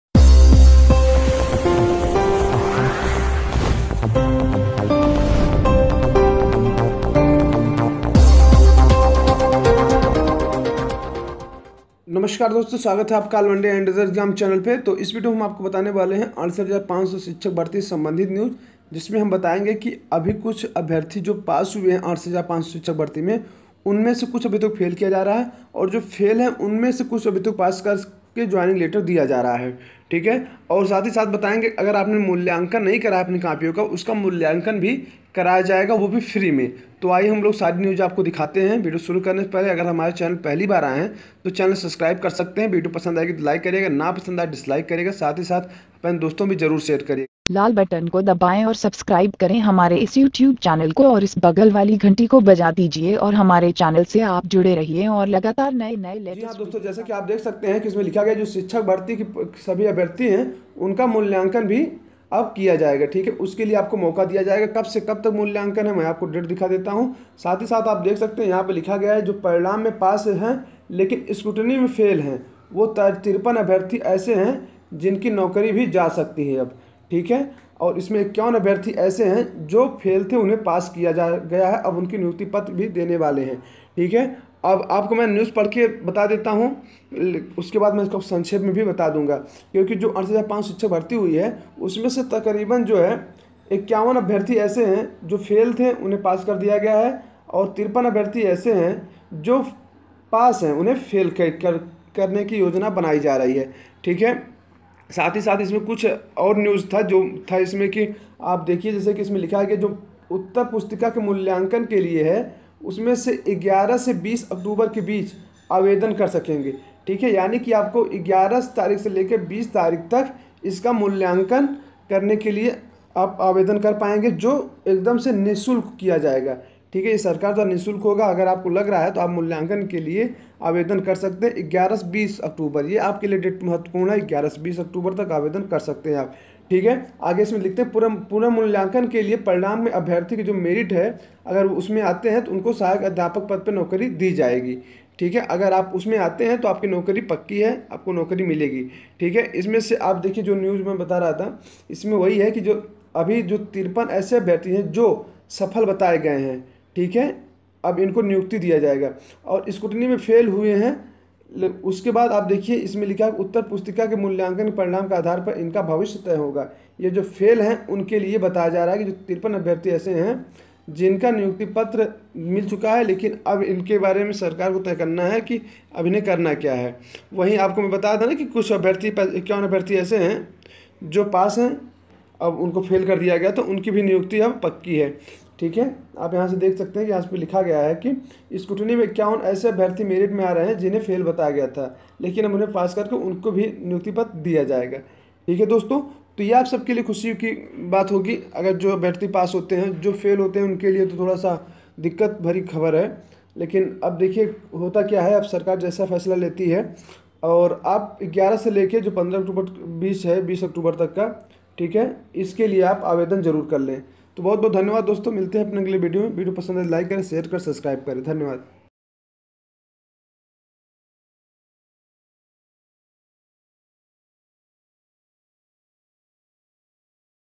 68500 शिक्षक भर्ती : फेल अभ्यर्थियों की जाएगी नौकरी।सभी अभ्यर्थियों को मिलेगा पुर्नमूल्यांकन का मौका
All oneday and other exam